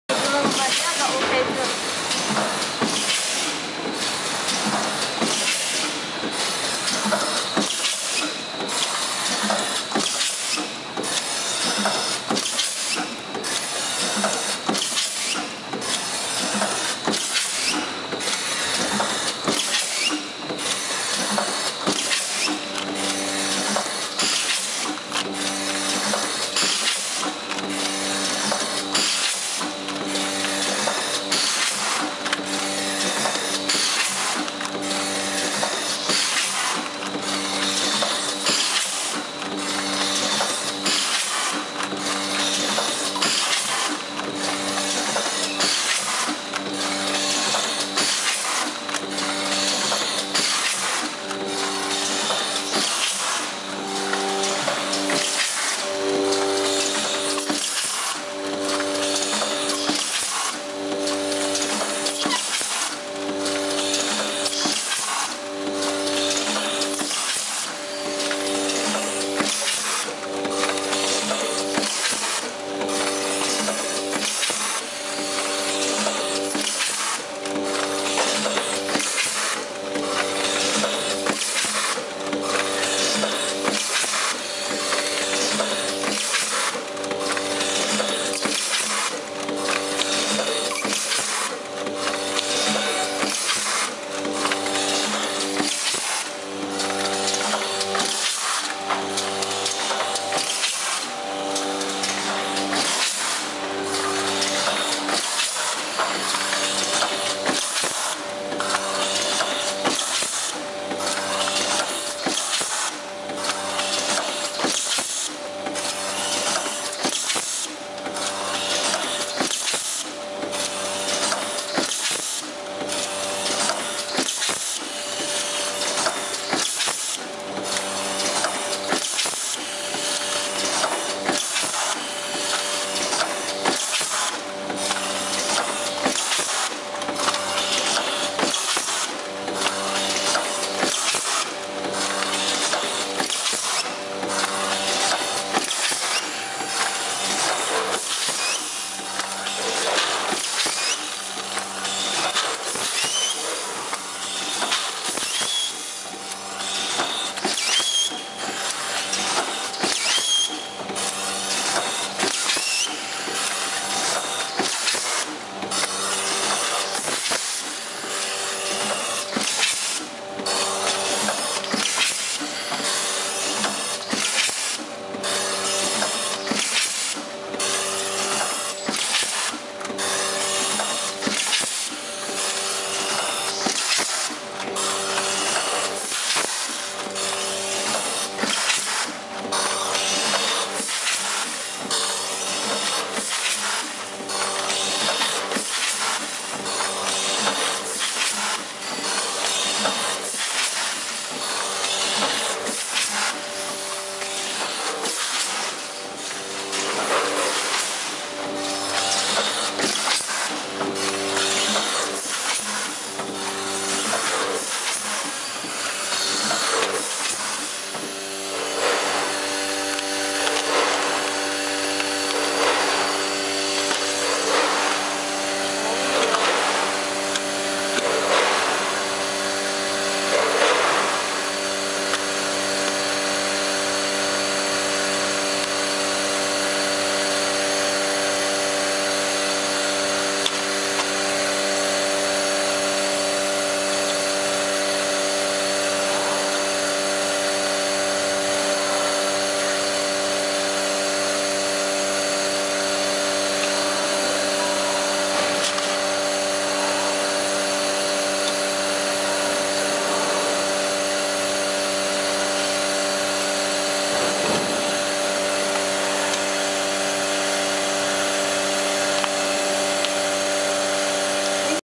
Automatic Premade bag packing machine with multiheads weigher
The automatic rotary packing machine adopts precision indexing device and PLC to control each action and working station to make sure the machine is easy to operate and doing accurately.

- The speed of this machine is controlled by a frequency converter. So that It can adjust according to the specific material and pouch which you need to pack.

- The bag width, length, and type can be changed according to your demands in the same machine. Changing different bags and different size is easy. (If you have different bag size and bag type, please consult your Contact Person). Adjust size by one handle but button so that it can easily operate and fix.

- The machine adopts safety door system, waster washing system, alarm system. If somebody opens the machine door when the machine is running, the machine will alarm and stop. If the air pressure shows abnormal, the machine will stop and alarm. (Any other alarm can check by the machine specification)

- An automatic checking system can check bag situation, filing, and sealing situation. The system show —— 1: no bag feeding, no filling, and no sealing —— 2: no bag opening/open error, will no filling and no sealing
—— 3: no filling, will no sealing

- The packing materials loss low, what this machine is used the preformed bag pattern is perfect and has a high quality of the sealing part, this improved product specification.

- Product or packing bag contact parts adopt stainless steel or other materials which accord with the food hygienic requirements,
guarantee hygiene and security of the food.